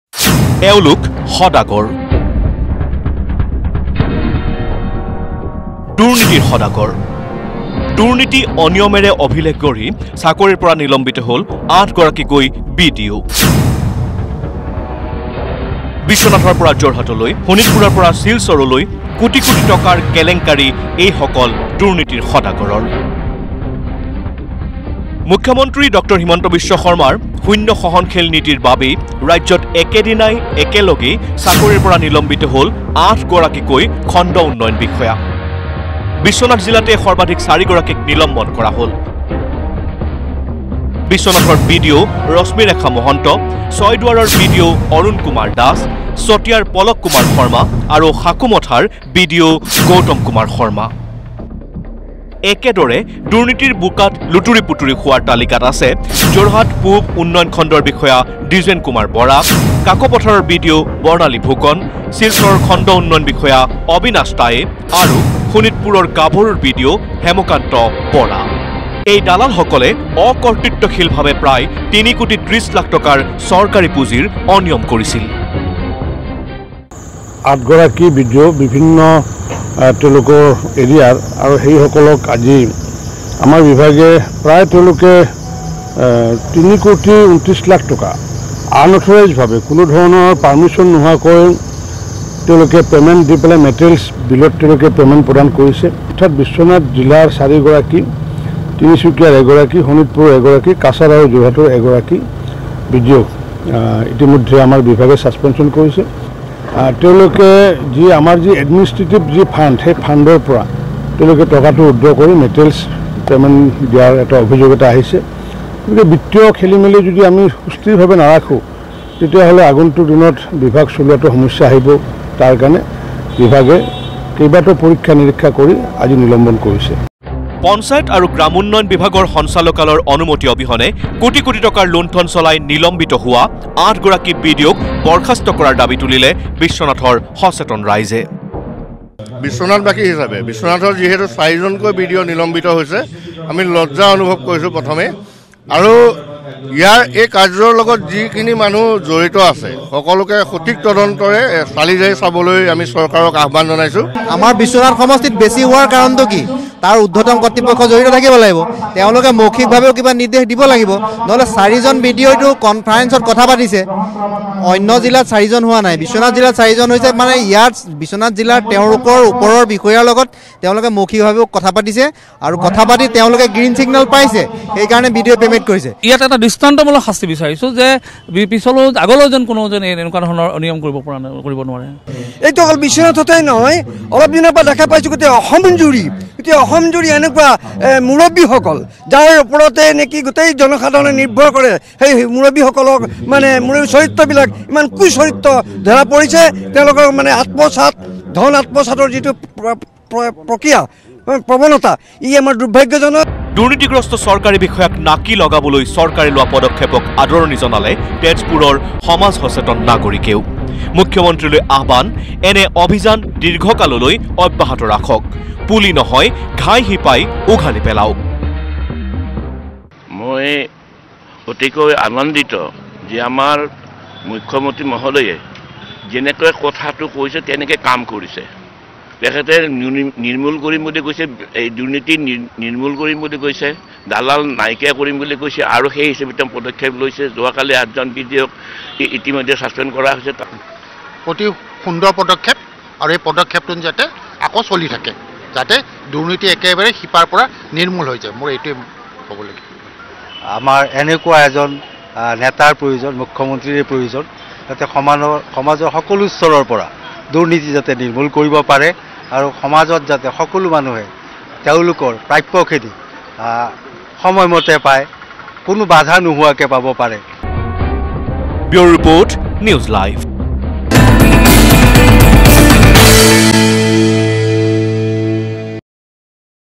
মুখ্যমন্ত্ৰীৰ শূন্য সহনশীল নীতিলৈ আদৰণি ৰাইজৰ, দুৰ্নীতিগ্ৰস্ত ৮ BDOক একেদিনাই নিলম্বন
📌মুখ্যমন্ত্ৰীৰ শূন্য সহনশীল নীতিলৈ আদৰণি ৰাইজৰ। দুৰ্নীতিগ্ৰস্ত ৮ BDOক একেদিনাই নিলম্বন। অকতৃত্বশীলভাবে মোকলাই নিছিল চৰকাৰী ধন। নিলম্বন নহয়, বৰ্খাস্তৰ দাবী ৰাইজৰ।